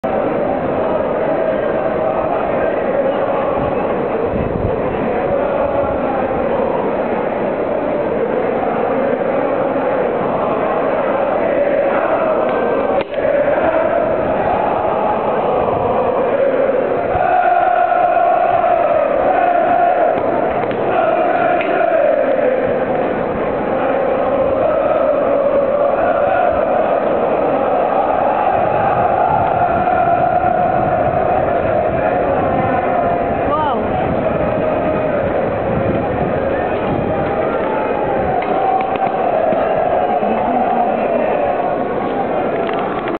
Spurs fans singing at Piazza Duomo in Milan.AVI
Spurs fans singing at Piazza Duomo in Milan